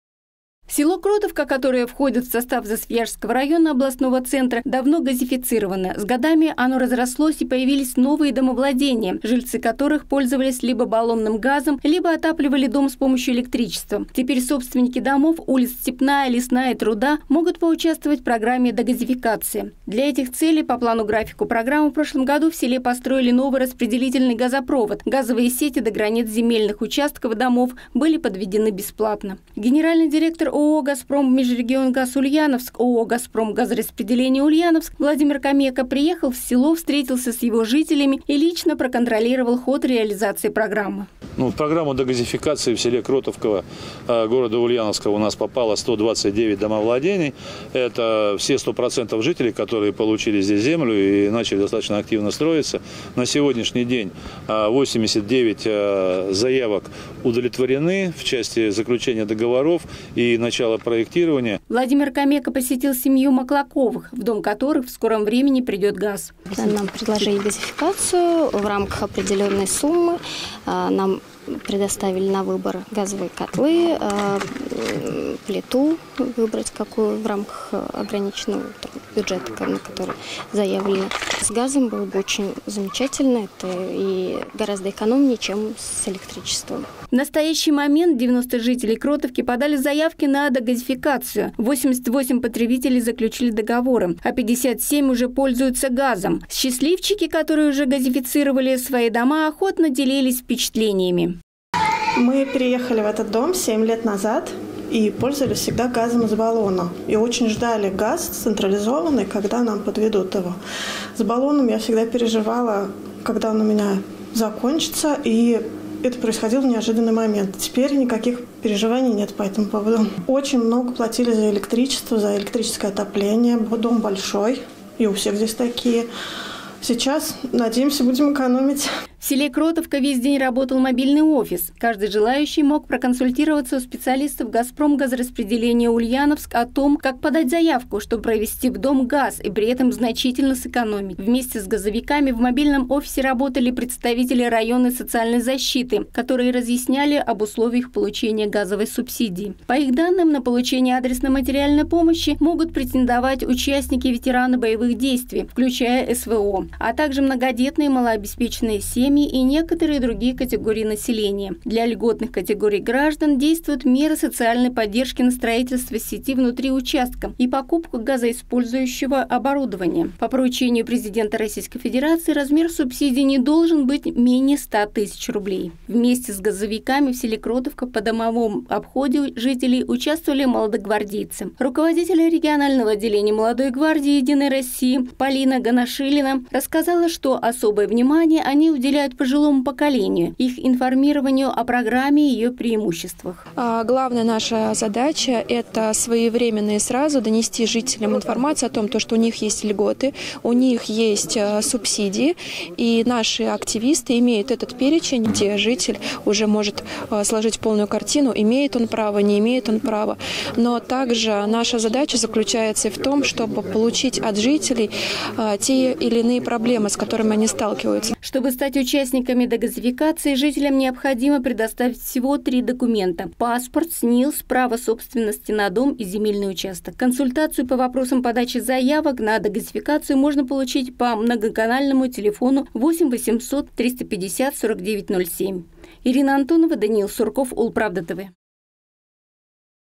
Догазификация — выгодно и просто. Ульяновские газовики и «единороссы» посетили жителей Кротовки
В Ульяновской области высокими темпами реализуется программа догазификации. Для её разъяснения «Газпром газораспределение Ульяновск» совместно с единороссами проводят встречи с населением в районах области. На одной из таких встреч побывала наша съёмочная группа.